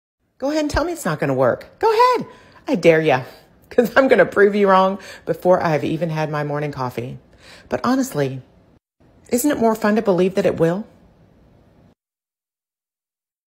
Yes! I Painted my ugly driveway. Whatcha gonna do about it?
Never tell a diyer that it can’t be done.